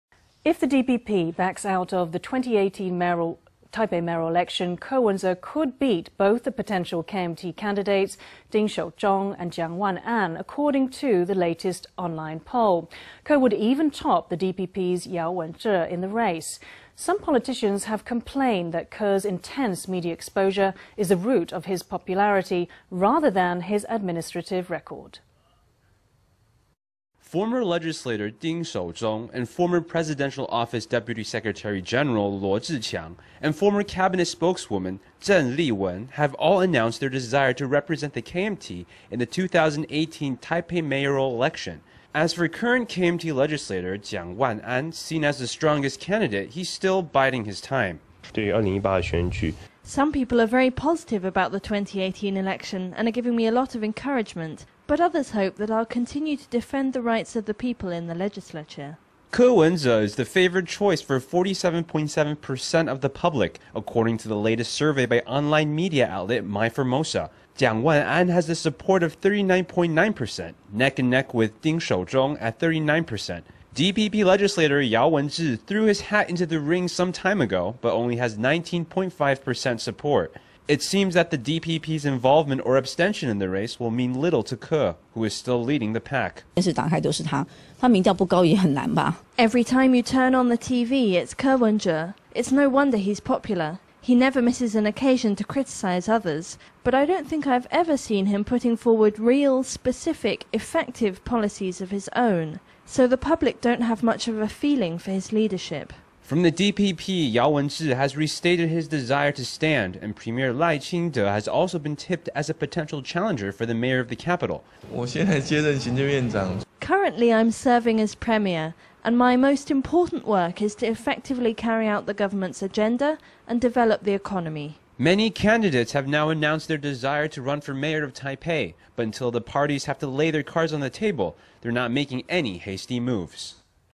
Ko Wen-je still most popular candidate for Taipei mayoral election 2018
If the DPP backs out of the 2018 Taipei mayoral election, Ko Wen-je could beat both the potential KMT candidates Ting Shou-chung and Chiang Wan-an, according to the latest online poll. Ko would even top the DPP''s Yao Wen-chih in the race. Some politicians have complained that Ko''s intense media exposure is the root of his popularity, rather than his administrative record.Former legislator Ting Shou-chung and former Presidential Office Deputy Secretary-General Lo Chih-chiang, and former Cabinet spokeswoman Cheng Li-wun have all announced their desire to represent the KMT in the 2018 Taipei mayoral election. As for current KMT legislator, Chiang Wan-an, seen as the strongest candidate – he’s still biding his time.Chiang Wan-anKMT LegislatorSome people are very positive about the 2018 election, and are giving me a lot of encouragement. But others hope that I will continue to defend the rights of the people in the legislature.Ko Wen-je is the favoured choice for 47.7% of the public, according to the latest survey by online media outlet My Formosa. Chiang Wan-an has the support of 39.9%, neck and neck with Ting Shou-chung at 39.0%. DPP Legislator Yao Wen-chih threw his hat into the ring some time ago, but only has 19.5% support. It seems that the DPP’s involvement or abstention in the race will mean little to Ko, who is still leading the pack.Hsu Shu-huaTaipei City CouncilorEvery time you turn on the TV it’s Ko Wen-je. It’s no wonder he’s popular. He never misses an occasion to criticize others. But I don’t think I’ve ever seen him putting forward real, specific, effective, policies of his own. So the public don’t have much of a feeling for his leadership.From the DPP, Yao Wen-chih has restated his desire to stand, and Premier Lai Ching-te has also been tipped as a potential challenger for the mayor of the capital.Lai Ching-tePremierCurrently I’m serving as Premier, and my most important work is to effectively carry out the government’s agenda and develop the economy.Many candidates have now announced their desire to run for Mayor of Taipei, but until the parties have to lay their cards on the table, they’re not making any hasty moves.